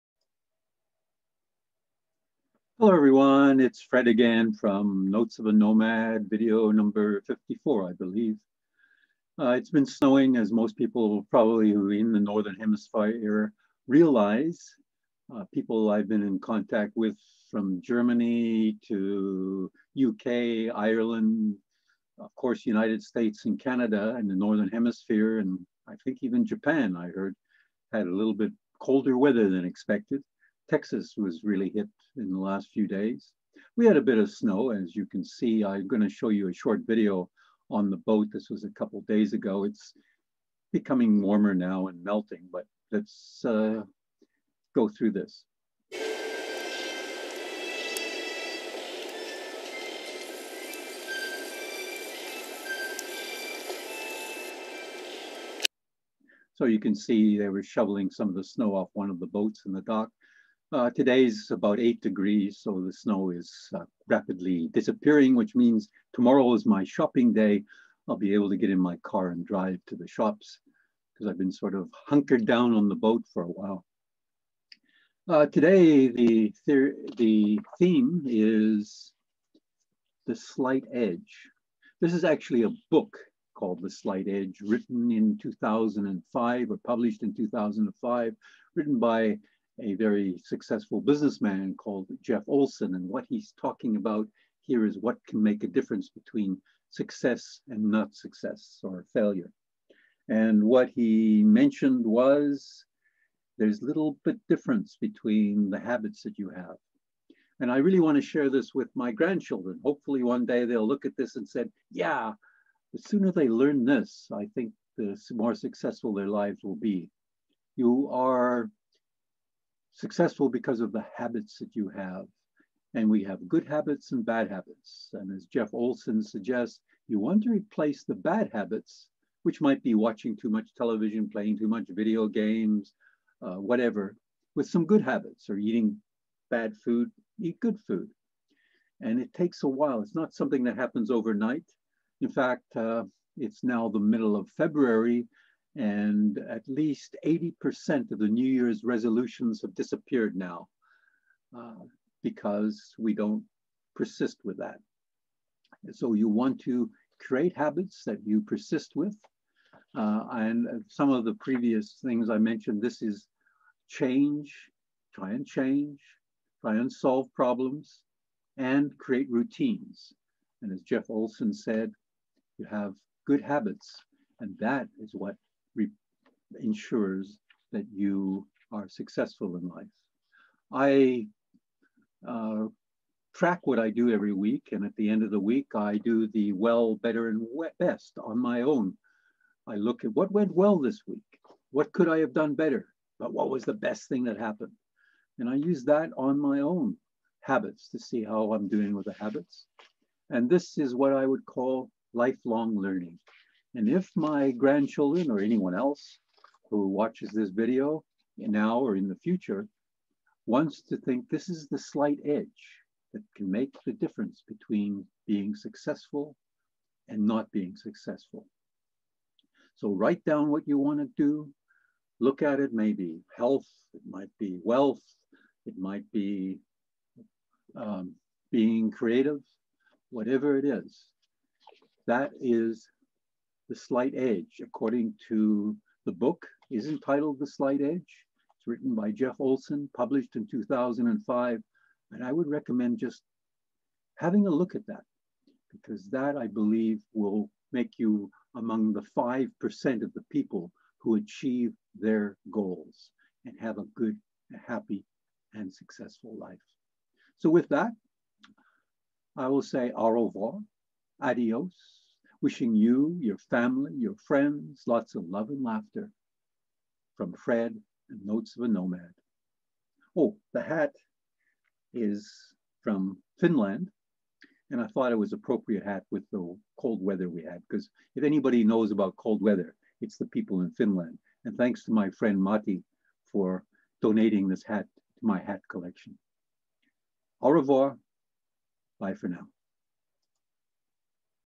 NOTES of a NOMAD #54 -The Slight Edge -16 February 2021
There has been lots of cold weather including snow throughout the northern hemisphere this past week. I share the power of small choices when we are able to change some bad habits to good habits.  This is what can lead to success. (This tip is especially for my grandchildren.)